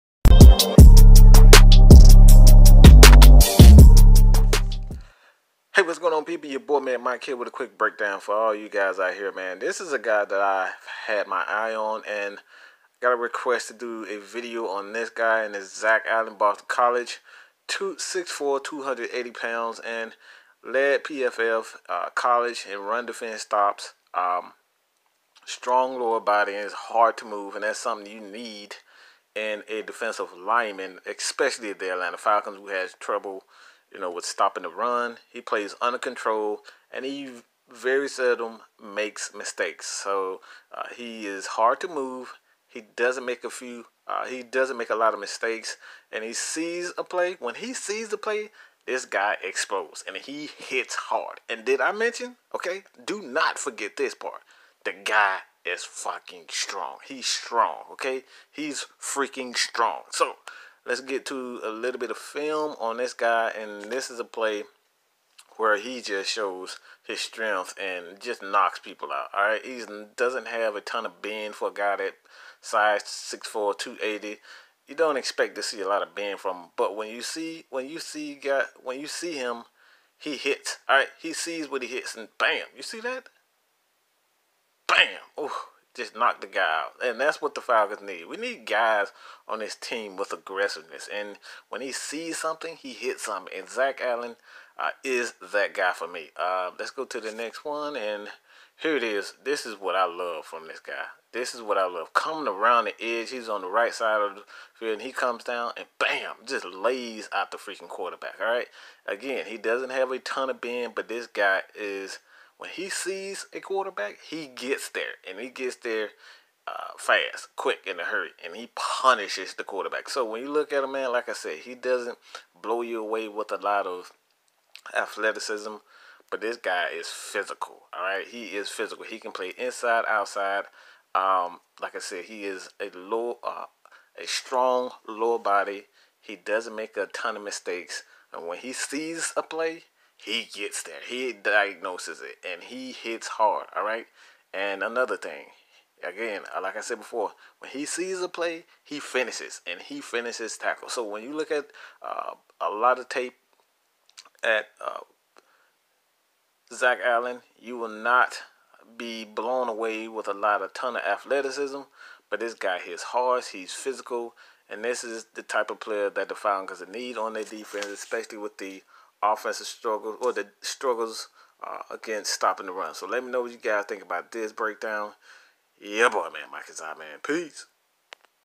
Atlanta Falcons Draft 2019 | Boston College Zach Allen
Atlanta Falcons Draft 2019 prospect Zach Allen is the next guy up. In this video Mad Mike gives his take on the Boston College Prospect.